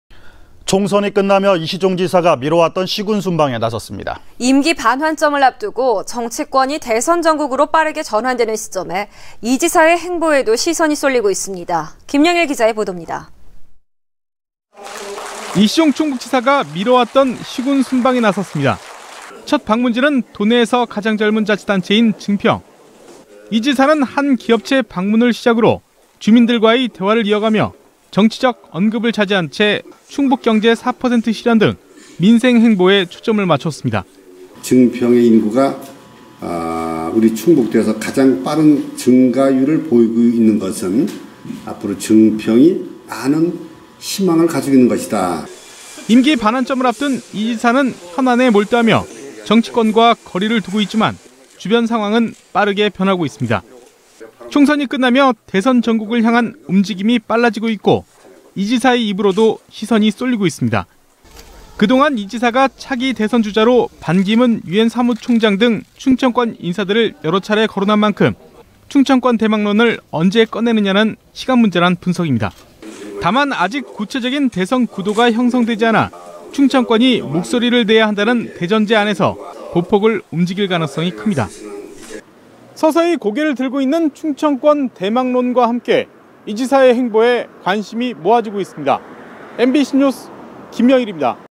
[청주MBC뉴스] 시.군 순방.. 충북지사 정치 행보
◀앵커▶
 총선이 끝나며 이시종 지사가 미뤄왔던
시군 순방에 돌입했습니다. 임기 반환점을
앞두고 정치권이 대선 정국으로 빠르게
전환되는 시점에 이 지사의 행보에도
시선이 쏠리고 있습니다. 김영일 기자입니다.
           ◀ＥＮＤ▶

           ◀ＶＣＲ▶
 이시종 충북지사가 미뤄왔던
시,군 순방에 나섰습니다.

 첫 방문지는 도내에서
가장 젊은 자치단체인 증평,

 이 지사는 한 기업체 방문을 시작으로
주민들과의 대화를 이어가며 정치적 언급을
자제한 채 충북경제 4% 실현 등 민생 행보에
초점을 맞췄습니다.

           ◀ＳＹＮ▶
        이시종/충북지사
    "증평 현안 관심갖고 있다"

 임기 반환점을 앞둔 이 지사는 현안에
몰두하며 정치권과 거리를 두고 있지만
주변 상황은 빠르게 변하고 있습니다.

 총선이 끝나며 대선 정국을 향한
움직임이 빨라지고 있고 이 지사의 입으로도
시선이 쏠리고 있습니다.

 그동안 이 지사가 차기 대선주자로
반기문 유엔 사무총장 등 충청권 인사들을
여러차례 거론한 만큼 충청권 대망론을
언제 꺼내느냐는 시간 문제란 분석입니다.

 다만, 아직 구체적인 대선 구도가 형성되지
않아 충청권이 목소리를 내야 한다는 대전제
내에서 보폭을 움직일 가능성이 큽니다.

(S/U)
 충청권 대망론이 서서히 고개를 들고 있는
가운데 이 지사의 행보에 점점 관심이
쏠리고 있습니다. MBC뉴스 김영일입니다."